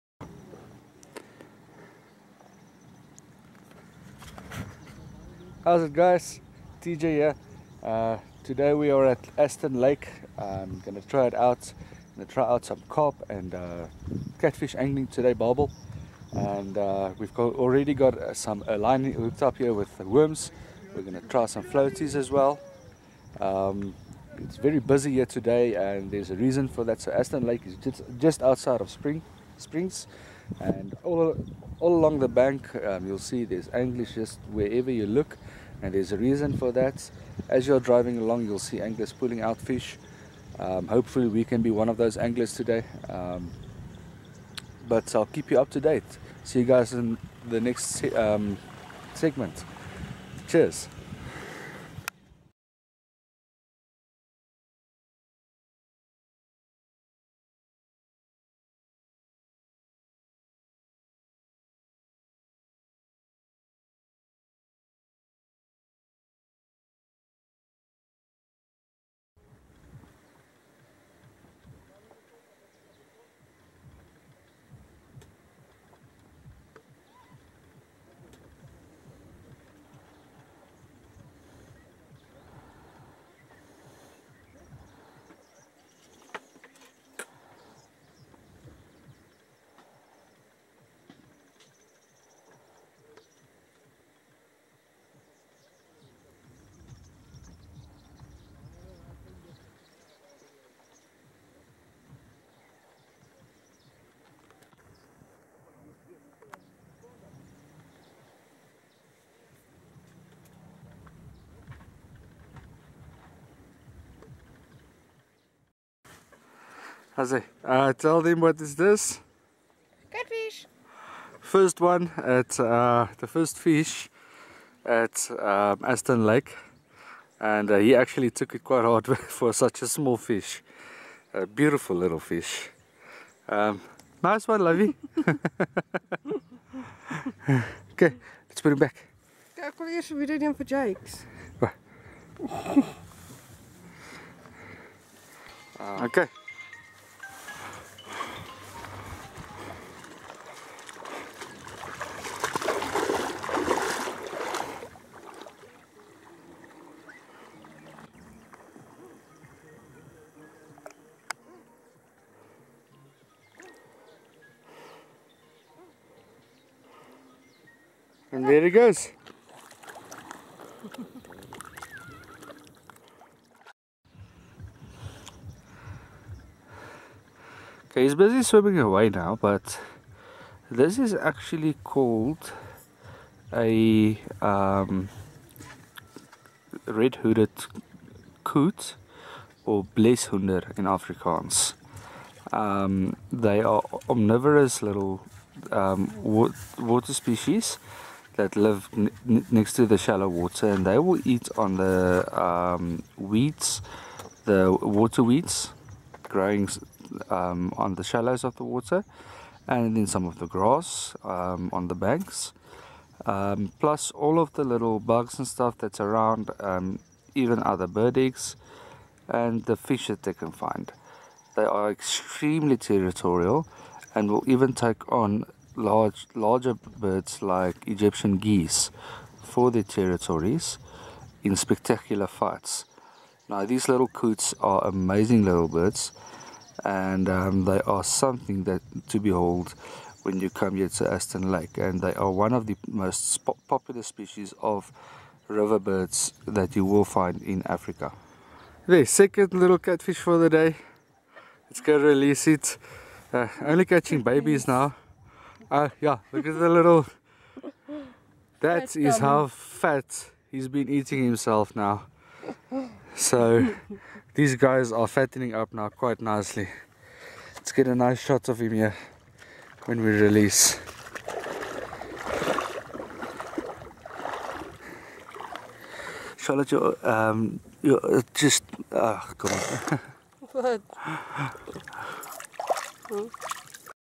Got humiliated by my wife at Aston Lake Springs
I completely blanked out catching not one fish but my wife caught two beautiful baby sharptooth catfish with full belly's